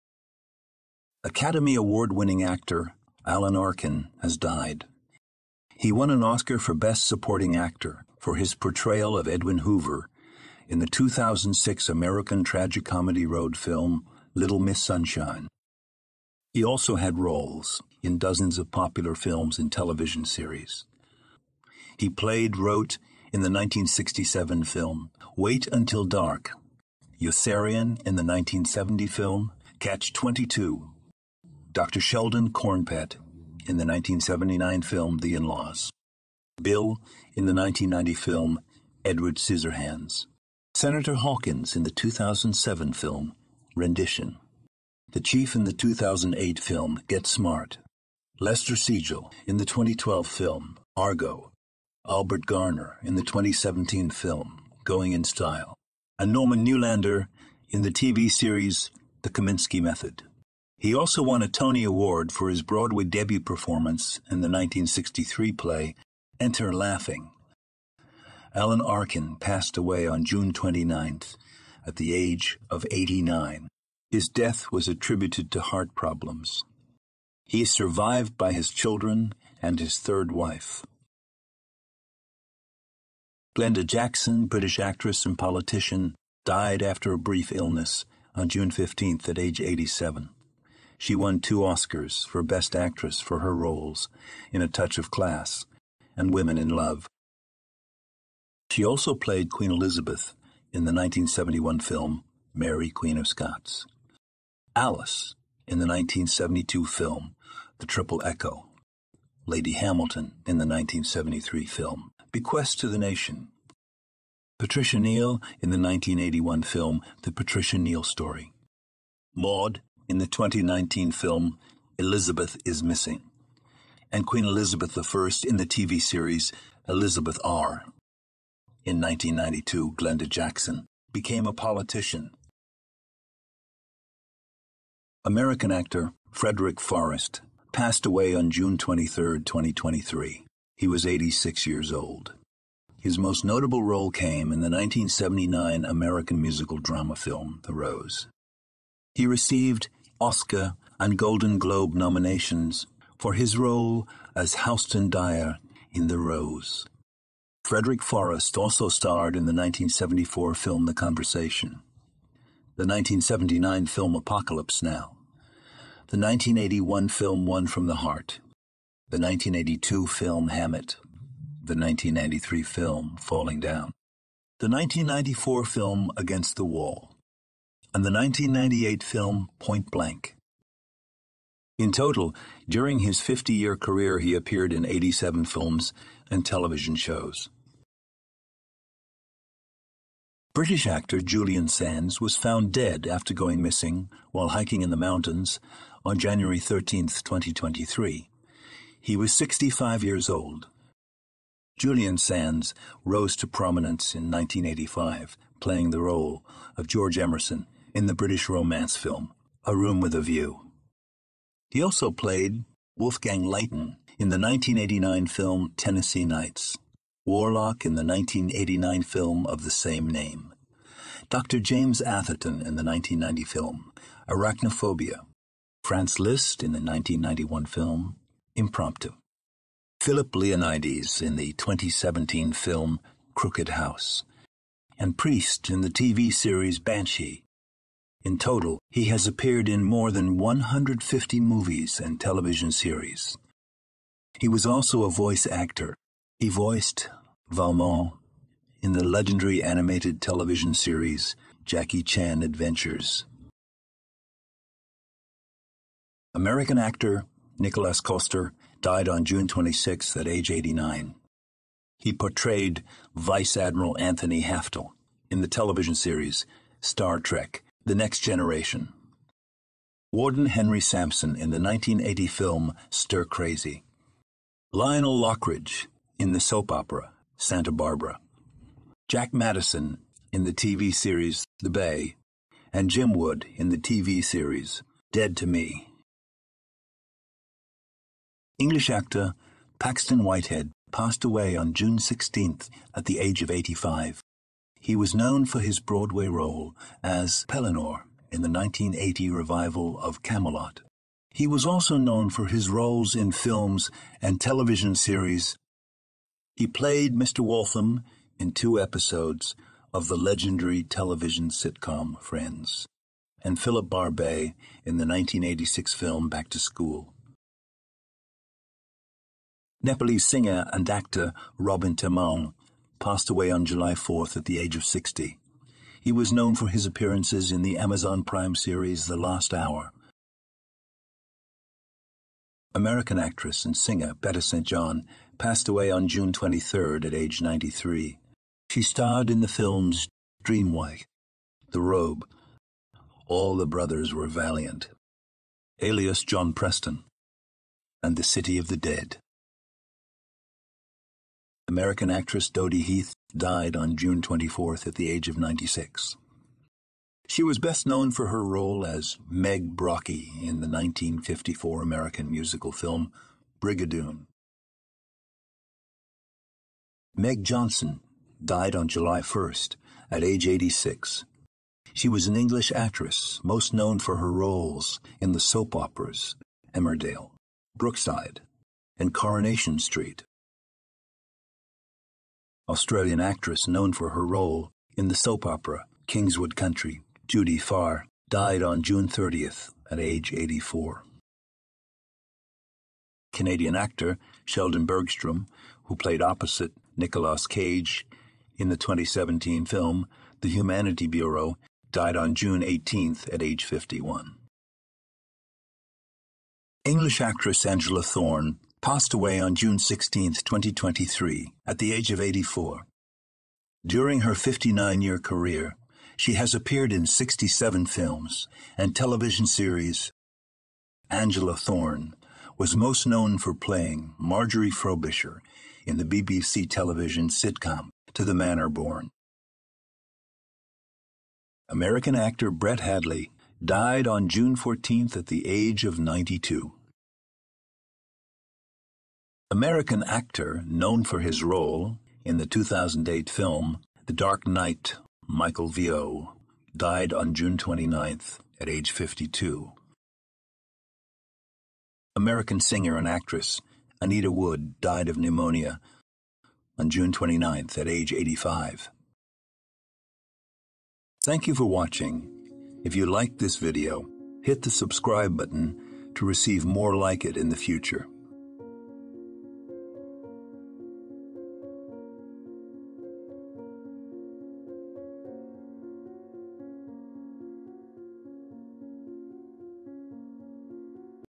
15 Famous Actors who recently passed away
Academy Award winning actor, Alan Arkin, has died. He won an Oscar for Best Supporting Actor, for his portrayal, of Edwin Hoover, in the 2006 American tragicomedy road film, Little Miss Sunshine. He also had roles, in dozens of popular film, and television series. He played, Roat, in the 1967 film, Wait Until Dark. Yossarian, in the 1970 film, Catch 22. Doctor Sheldon Kornpett, in the 1979 film, The InLaws. Bill, in the 1990 film, Edward Scissorhands. Senator Hawkins, in the 2007 film, Rendition. The Chief, in the 2008 film, Get Smart. Lester Siegel, in the 2012 film, Argo. Albert Garner, in the 2017 film, Going in Style. And Norman Newlander, in the TV series, The Kominsky Method. He also won a Tony Award, for his Broadway debut performance, in the 1963 play, Enter Laughing. Alan Arkin, passed away on June 29th, at the age of 89. His death was attributed to heart problems. He is survived by his children, and his third wife.
Glenda Jackson, British actress, and politician, died after a brief illness, on June 15th, at age 87. She won 2 Oscars, for  Best Actress, for her roles, in A Touch of Class. And Women in Love. She also played, Queen Elizabeth, in the 1971 film, Mary, Queen of Scots. Alice, in the 1972 film, The Triple Echo. Lady Hamilton, in the 1973 film, Bequest to the Nation. Patricia Neal, in the 1981 film, The Patricia Neal Story. Maud, in the 2019 film, Elizabeth Is Missing. And Queen Elizabeth the first, in the TV series, Elizabeth R. In 1992, Glenda Jackson, became a politician.
American actor, Frederic Forrest, passed away on June 23rd, 2023. He was 86 years old. His most notable role came, in the 1979 American musical drama film, The Rose. He received, Oscar, and Golden Globe nominations, for his role, as Houston Dyer, in The Rose. Frederic Forrest, also starred, in the 1974 film, The Conversation. The 1979 film, Apocalypse Now. The 1981 film, One from the Heart. The 1982 film, Hammett. The 1993 film, Falling Down. The 1994 film, Against the Wall. And The 1998 film, Point Blank. In total, during his 50-year career, he appeared in 87 films, and television shows.
British actor, Julian Sands, was found dead, after going missing, while hiking in the mountains, on January 13th, 2023. He was 65 years old. Julian Sands, rose to prominence in 1985, playing the role, of George Emerson, in the British romance film, A Room with a View. He also played, Wolfgang Leighton, in the 1989 film, Tennessee Nights. Warlock, in the 1989 film, of the same name. Doctor James Atherton, in the 1990 film, Arachnophobia. Franz Liszt, in the 1991 film, Impromptu. Philip Leonides, in the 2017 film, Crooked House. And Priest, in the TV series, Banshee. In total, he has appeared in more than 150 movies, and television series. He was also a voice actor. He voiced, Valmont, in the legendary animated television series, Jackie Chan Adventures.
American actor, Nicolas Coster, died on June 26th, at age 89. He portrayed, Vice Admiral, Anthony Haftel, in the television series, Star Trek: The Next Generation. Warden Henry Sampson, in the 1980 film, Stir Crazy. Lionel Lockridge, in the soap opera, Santa Barbara. Jack Madison, in the TV series, The Bay. And Jim Wood, in the TV series, Dead to Me.
English actor, Paxton Whitehead, passed away on June 16th, at the age of 85. He was known for his broadway role, as Pellinore, in the 1980 revival of Camelot. He was also known for his roles in films, and television series. He played, Mister Waltham, in 2 episodes, of the legendary television sitcom, Friends. And Philip Barbay, in the 1986 film, Back to School.
Nepalese singer, and actor, Robin Tamang, passed away on July 4th, at the age of 60. He was known for his appearances, in the Amazon Prime series, The Last Hour.
American actress, and singer, Betta St. John, passed away on June 23rd, at age 93. She starred in the films, Dream Wife. The Robe. All the Brothers Were Valiant. Alias John Preston. And The City of the Dead.
American actress, Dodie Heath, died on June 24th, at the age of 96. She was best known for her role, as Meg Brockie, in the 1954 American musical film, Brigadoon.
Canadian actor, Sheldon Bergstrom, who played opposite, Nicolas Cage, in the 2017 film, The Humanity Bureau, died on June 18th, at age 51.
English actress, Angela Thorne, passed away on June 16th, 2023, at the age of 84. During her 59 year career, she has appeared in 67 films, and television series. Angela Thorne, was most known for playing, Marjory Frobisher, in the BBC television sitcom, To the Manor Born.
American actor, Brett Hadley, died on June 14th, at the age of 92.
American actor, known for his role, in the 2008 film, The Dark Knight, Michael Vieau, died on June 29th, at age 52.
American singer, and actress, Anita Wood, died of pneumonia, on June 29th, at age 85.